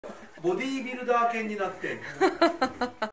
シェルティブルーマール ぷると3歳…ボディービルダー犬!!
シェルティブルーマールぷると 3歳 ムキムキ筋肉  ボディービルダー犬